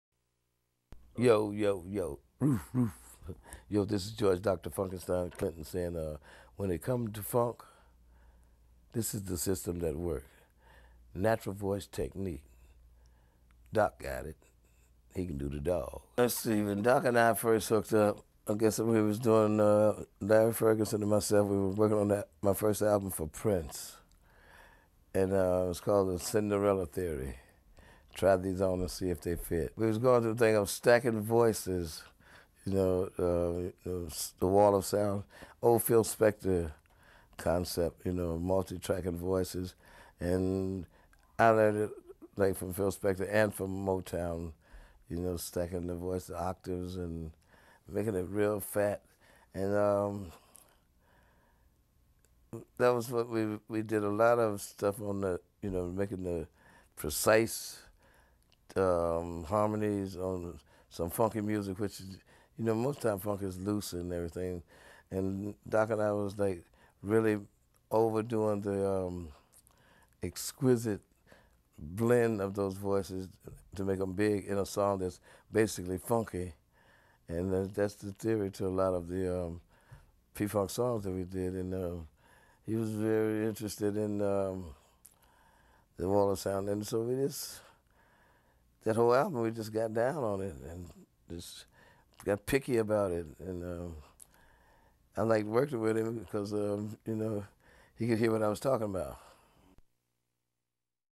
George Clinton Parliament Funkadelic - Interview
Legend George Clinton exclusive interview about another legend Doc Holliday, the singing guru.